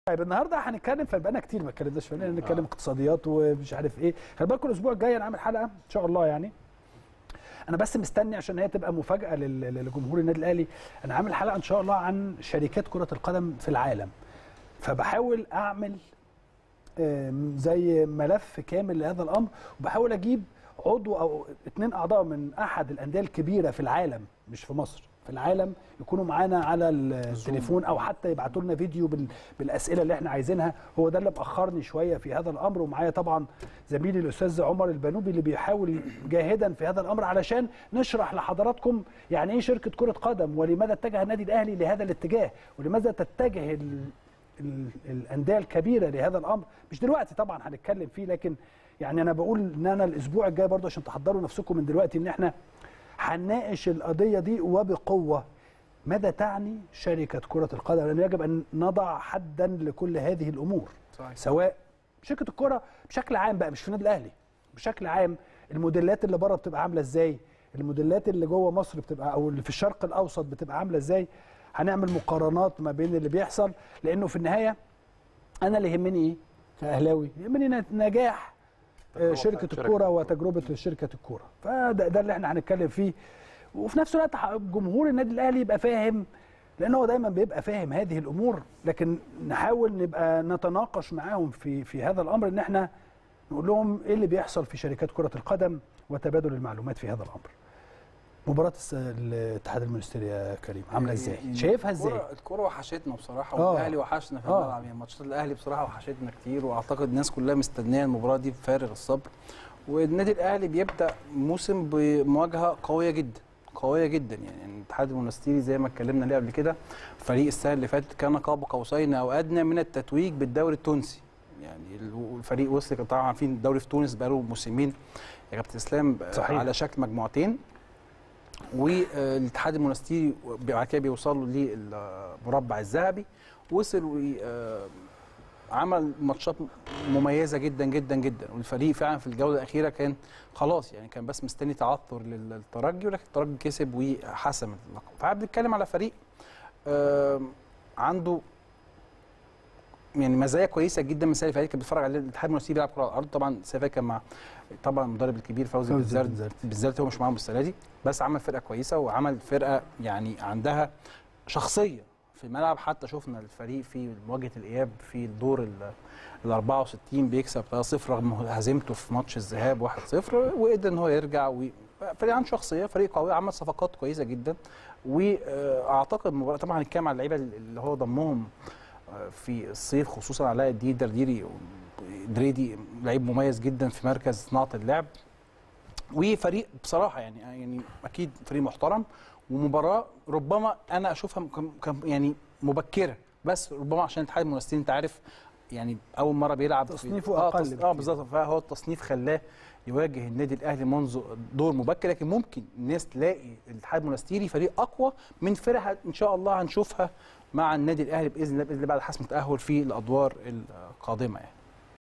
كريم سعيد: اتحاد المنستيري خصم قوي.. والمواجهات بين الأهلي والفرق التونسية تشهد ندية
كريم سعيد: اتحاد المنستيري خصم قوي.. والمواجهات بين الأهلي والفرق التونسية تشهد ندية | البيت الكبير